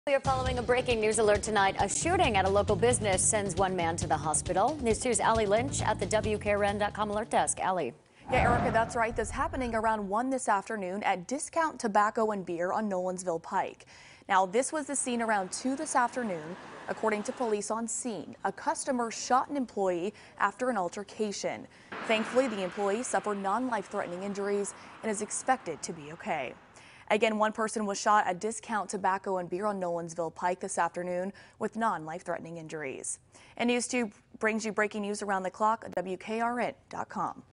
Customer accused of shooting employee at tobacco shop
Police are investigating following a shooting at a Nolensville Pike business.